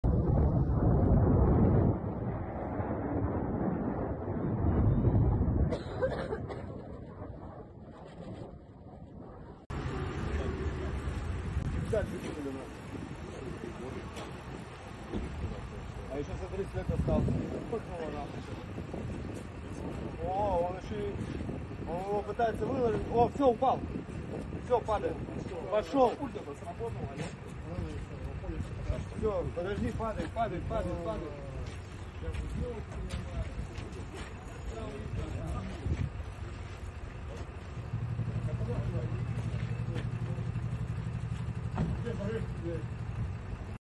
Russian MiG-31 Fighter Jet Goes Up in Flames Before Crash | Murmansk, Russia
A Russian MiG-31 fighter jet crashed in the northern Murmansk region on Wednesday, the TASS news agency reported, citing the defense ministry.

“The jet crashed in an uninhabited area,” it quoted the ministry as saying.

The pilots ejected and are safe, TASS added.

It did not say what had caused the fighter plane to crash.